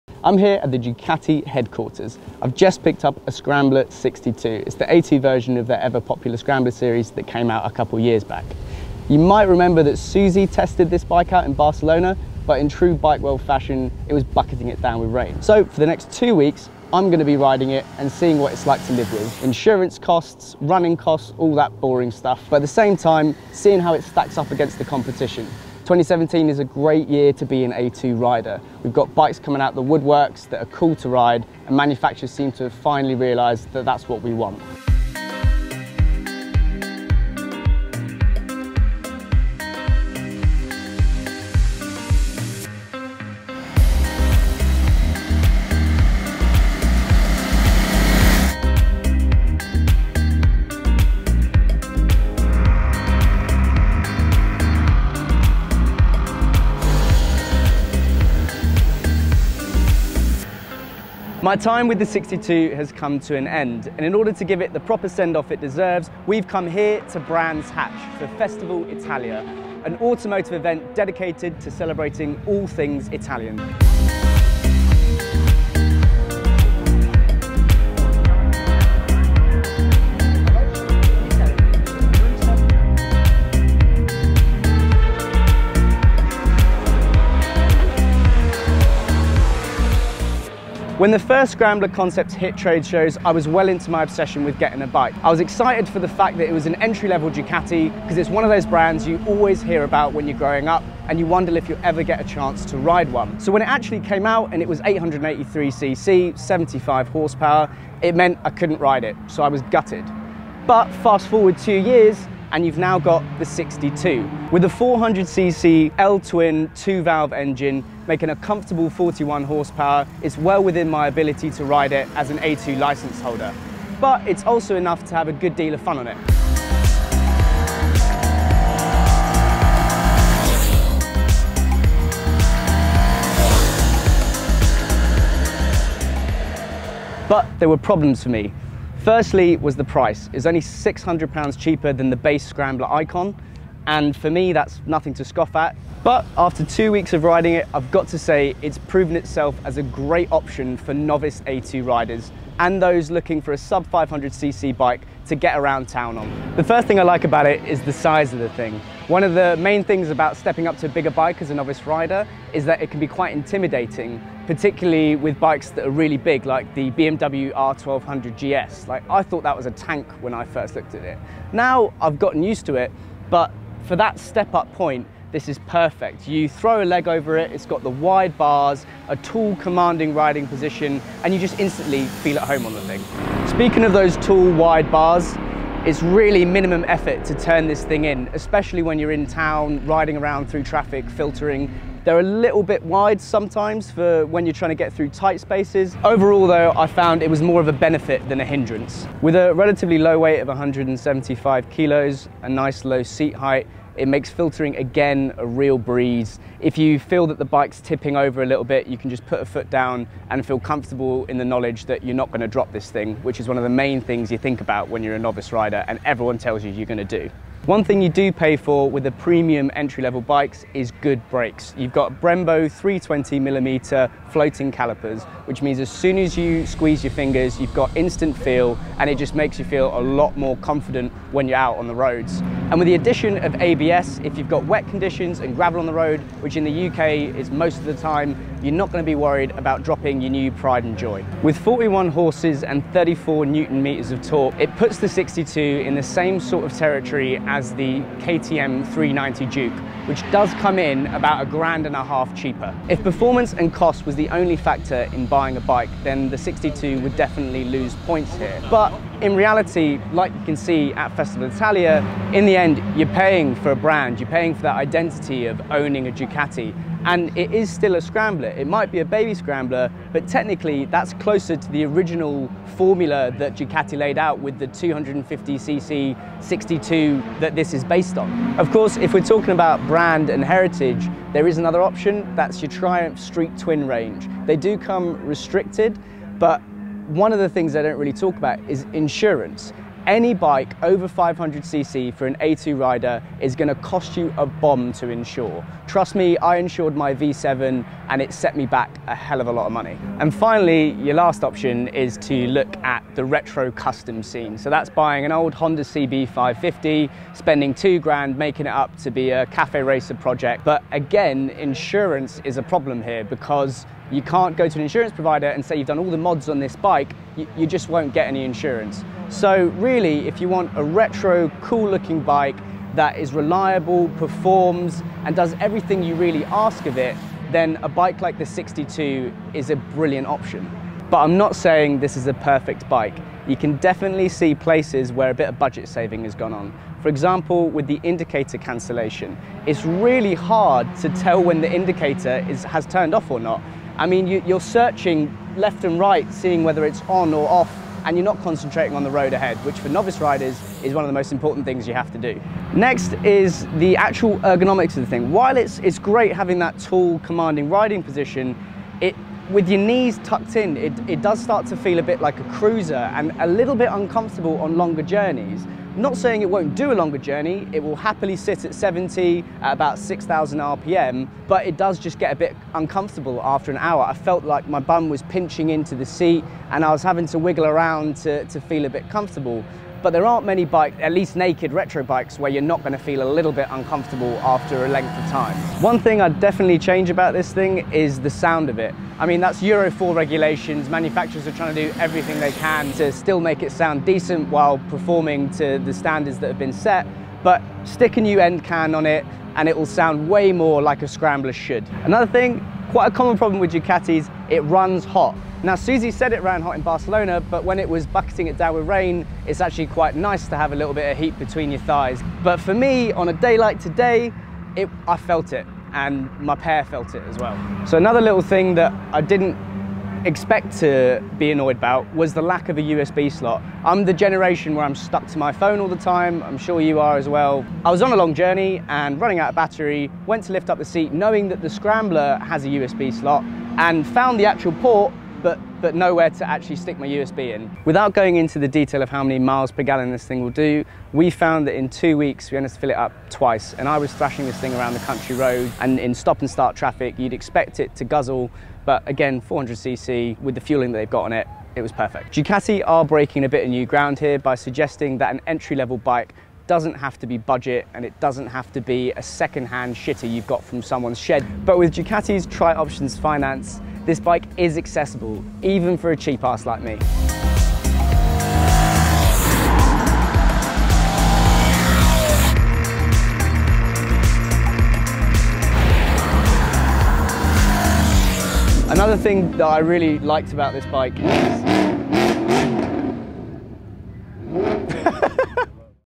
Ducati Scrambler Sixty2 A2 Rider Review
Our A2 license rider Alex spent a couple of weeks with the Scrambler Sixty2 to see how it stacks up against the A2 market.

Note: Slip of the tongue the Scrambler Icon is 803cc.